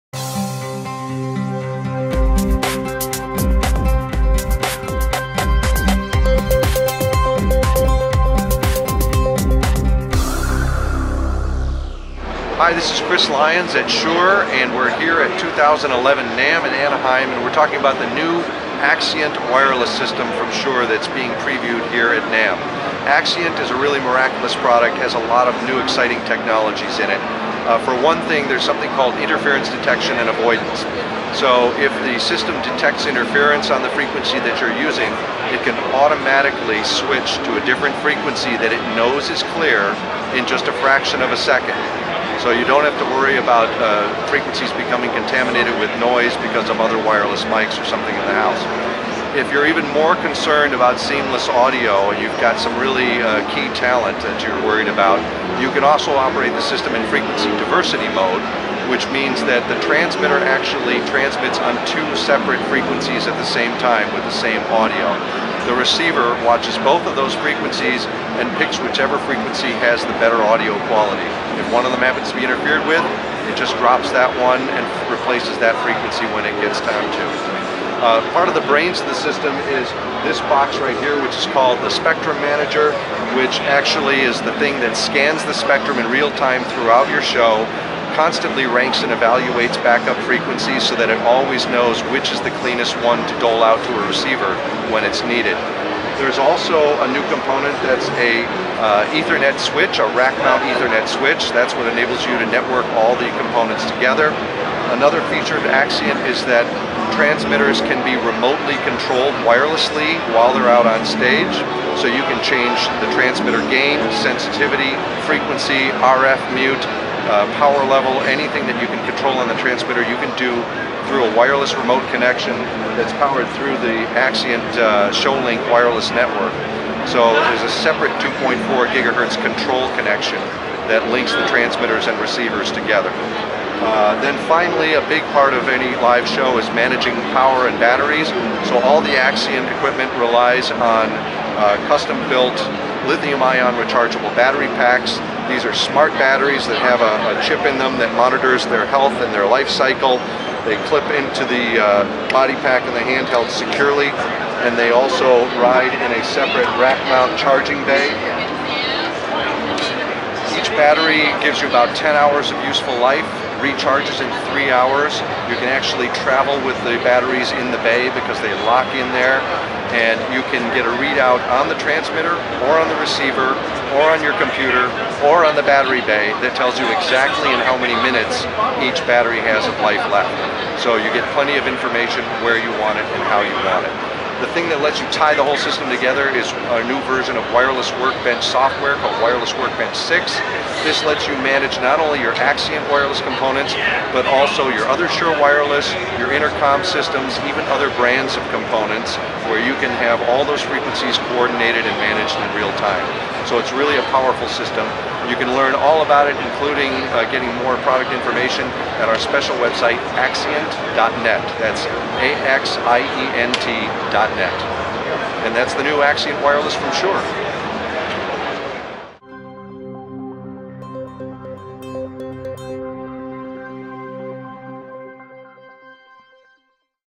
WNAMM 2011 Shure Axient Wireless System
Shure introduced two new wireless systems at the 2011 Winter NAMM show, one low-end, and one high-end. Axient is the high-end one, and it offers a level of sophistication that justifies the price tag.